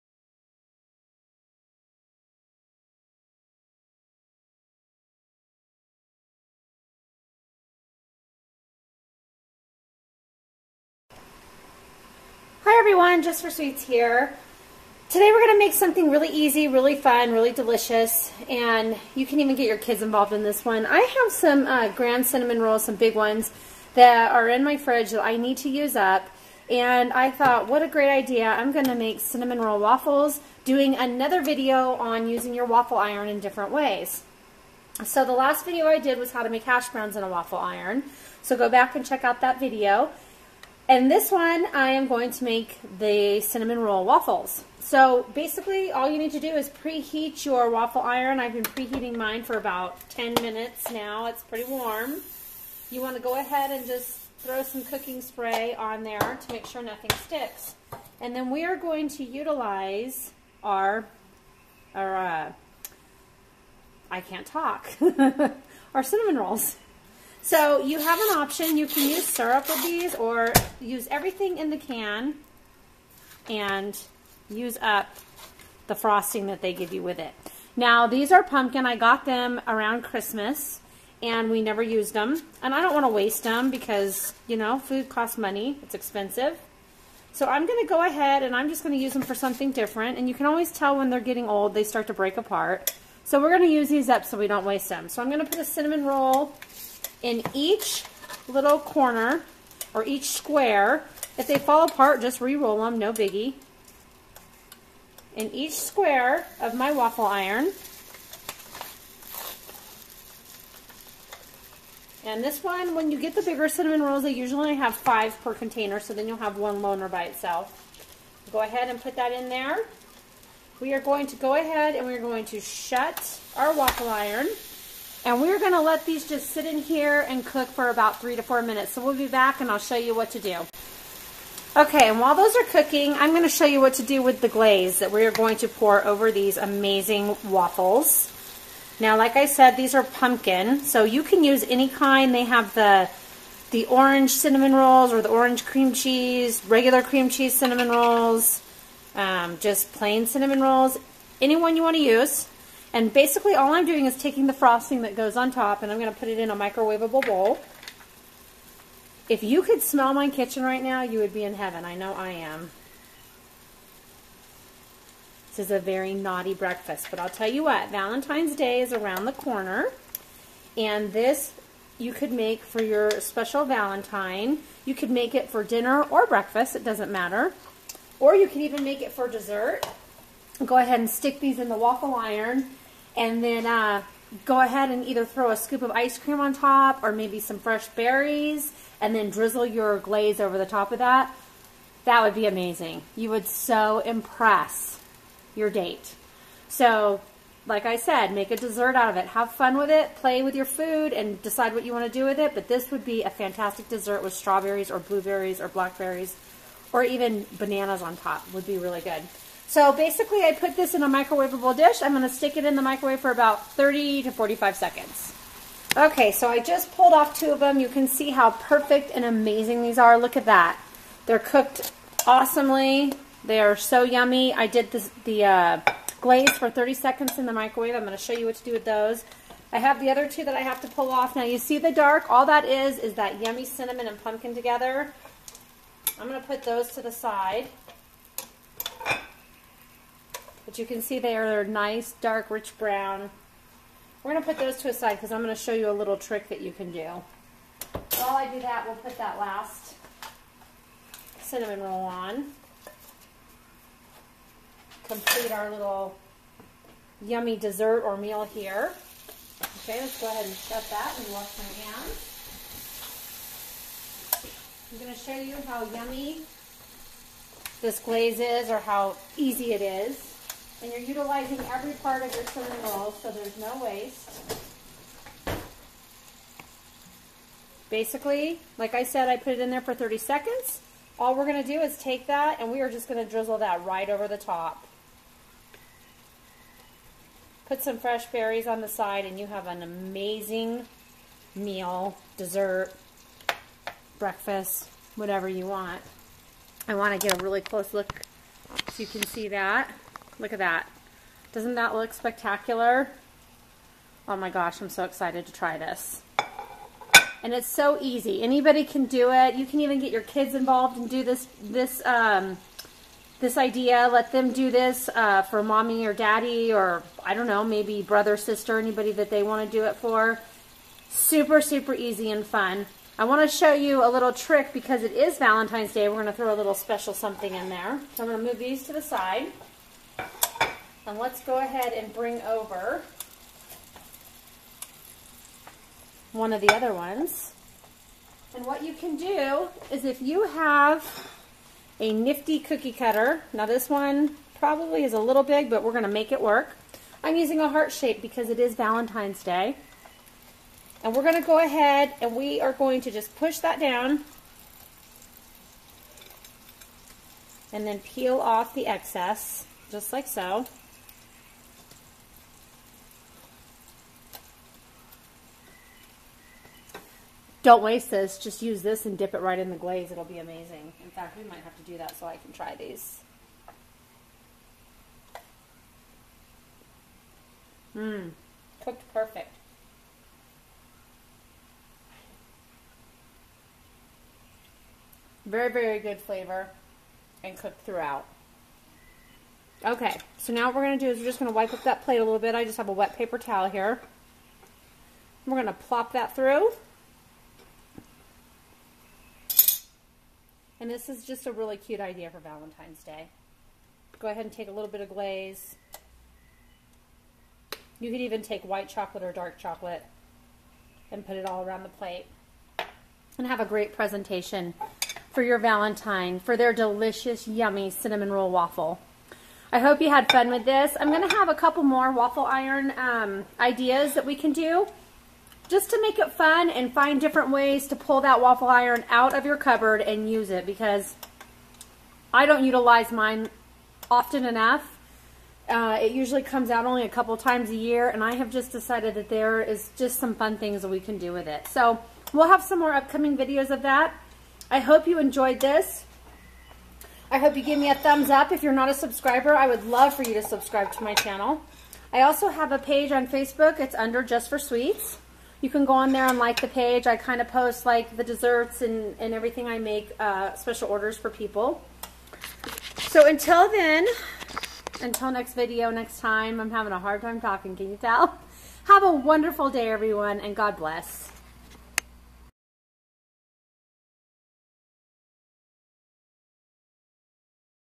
Easy Cinnamon Roll Waffles: How to utilize your waffle iron
This is another video in my waffle iron series. A fun way to utilize your waffle iron to create unique foods.
This one is great for your Valentine.
Makes a good meal or an even better dessert.
Check out my Hashbrowns waffle iron style video for another great idea.